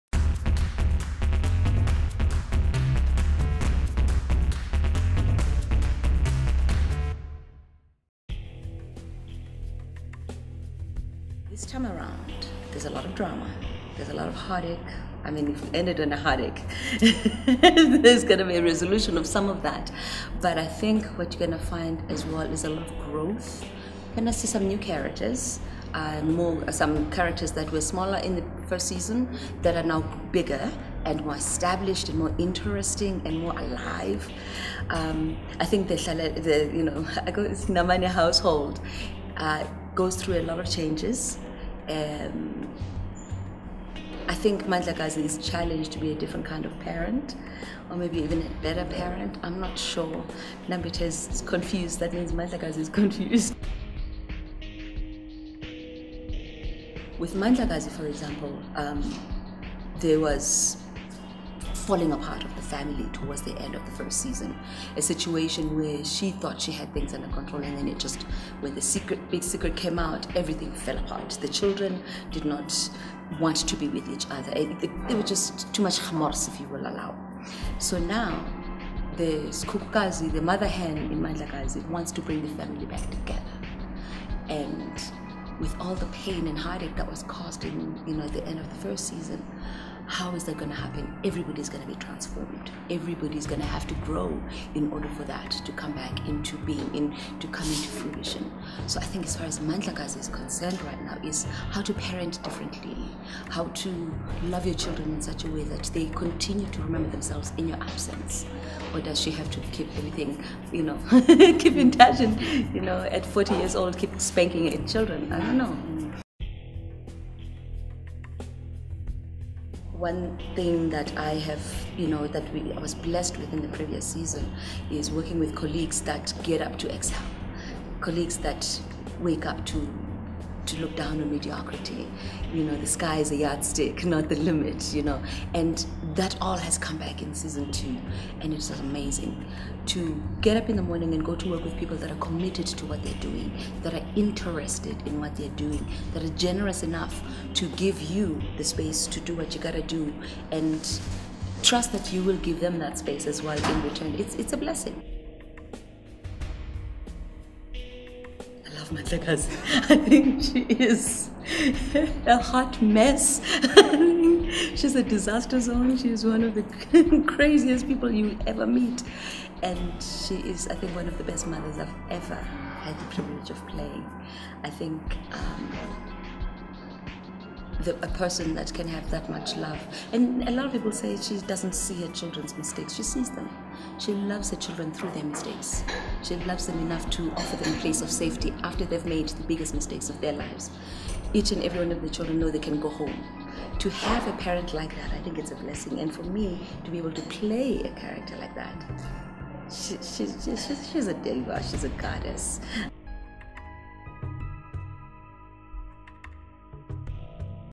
Nambitha Mpumlwana on Ashes to Ashes
Nambitha Mpumlwana aka Mandlakazi talks about her character on Ashes to Ashes Season 2. Starts Monday, 14 March 2016.